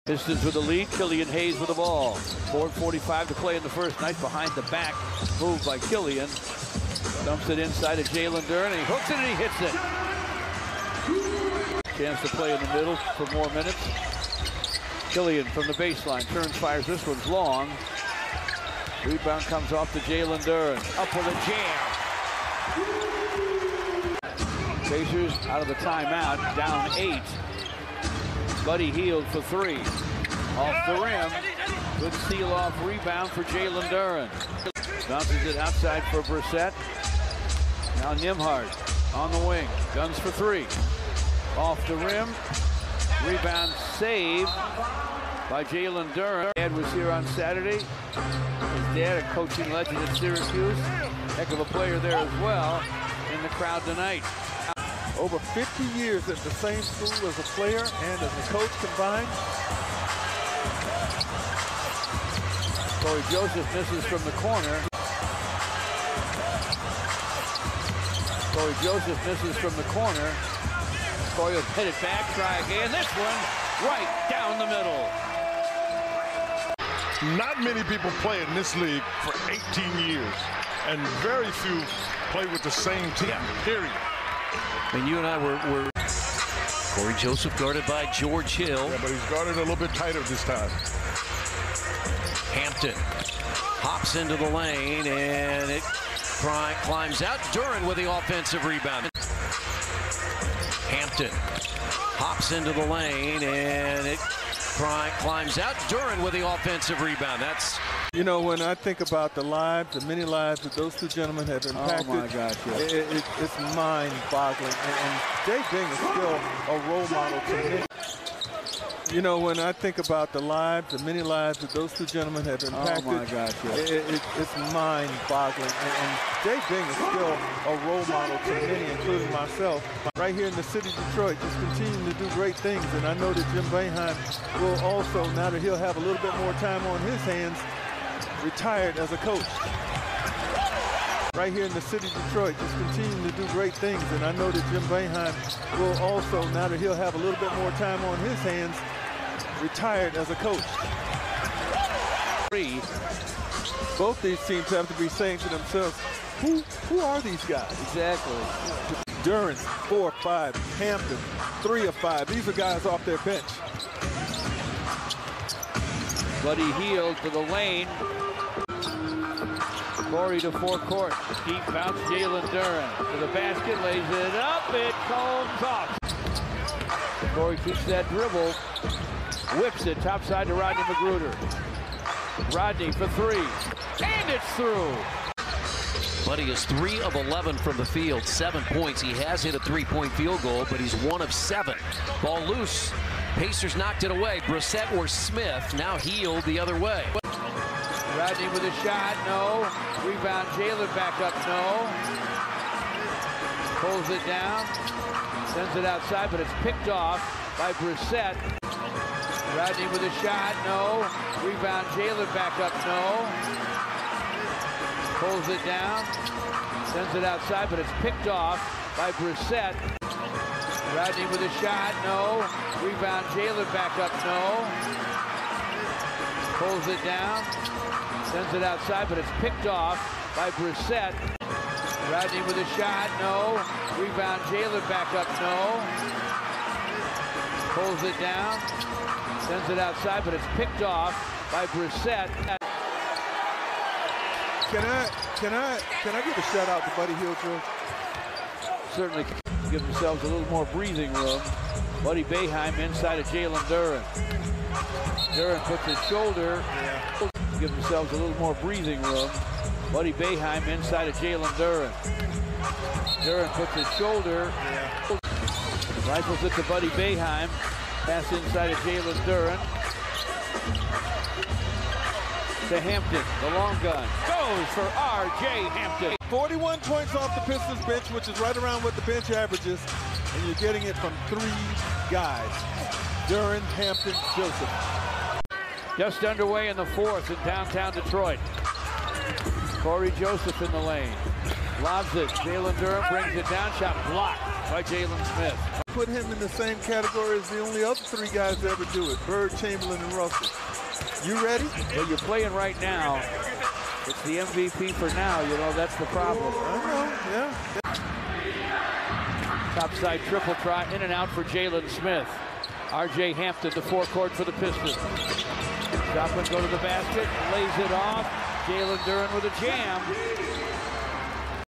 Jalen Duren 12 PTS 11 REB: All Possessions (2023-03-13)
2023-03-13 Detroit Pistons vs Indiana Pacers: Player Highlights Jalen Duren
    12 PTS, 11 REB, 3 AST, 1 STL.
        0 of 0 from 3.
        6 of 11 FGs in 23:32 minutes.